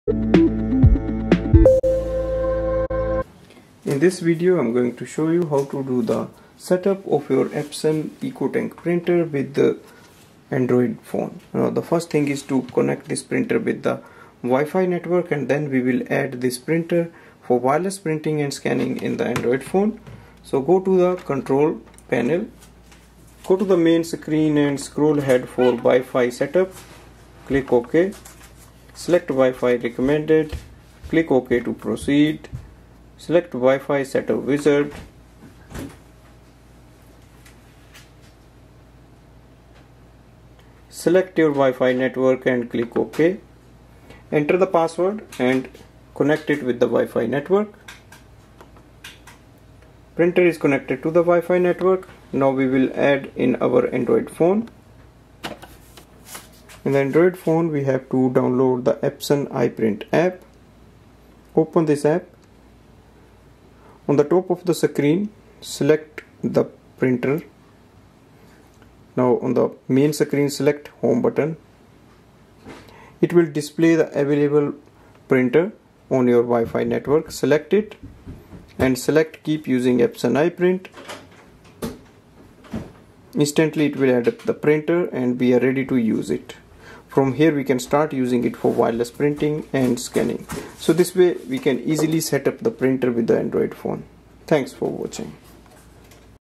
Epson EcoTank ET-2803 Setup Android Phone Review.
This video reviews the method to set up your Epson Eco Tank ET-2803 All-in-one Printer. This video shows how to do the Wireless WiFi setup of this printer and add it to an Android Phone.